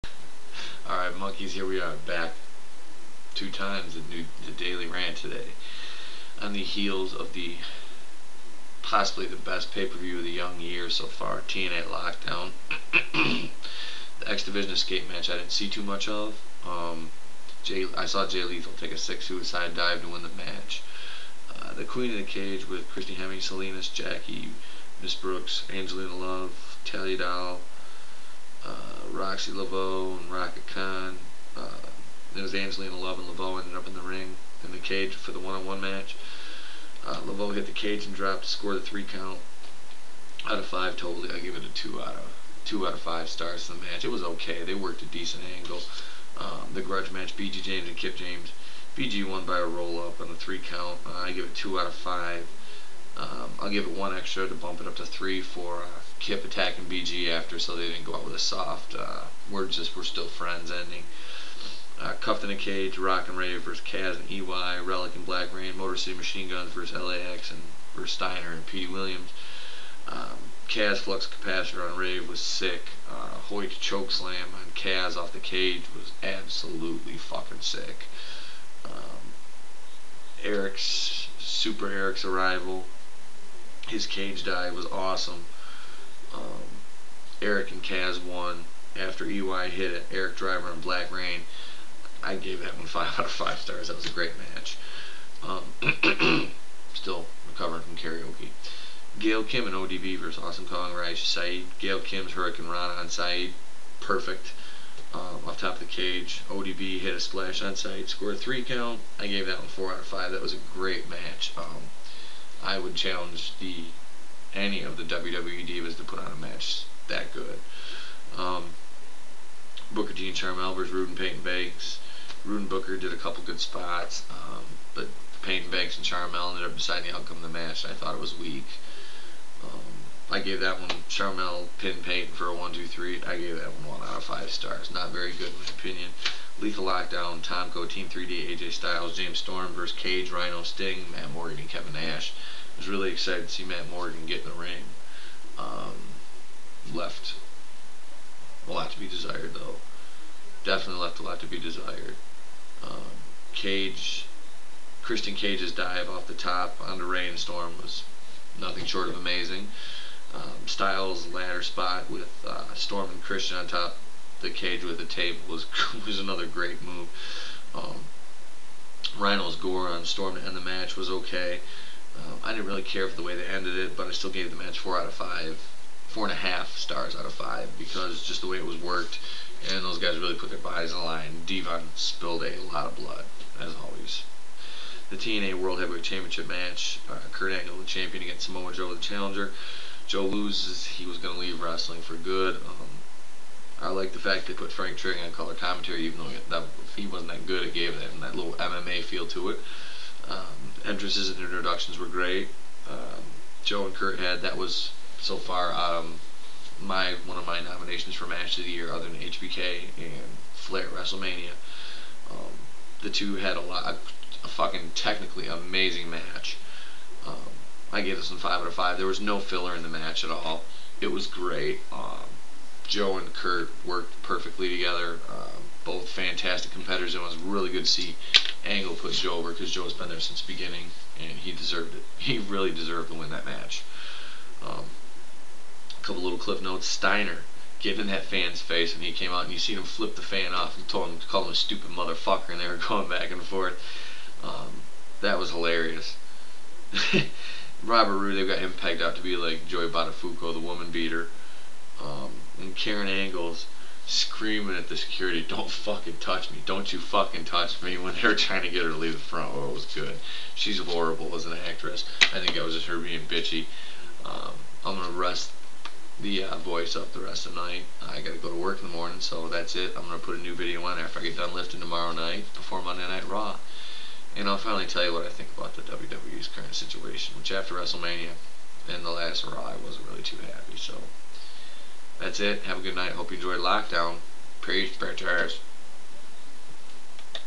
The Daily Rant 4/14/08
I review the SPECTACULAR 4.5 star out of 5 star Pay Per View that was TNA Lockdown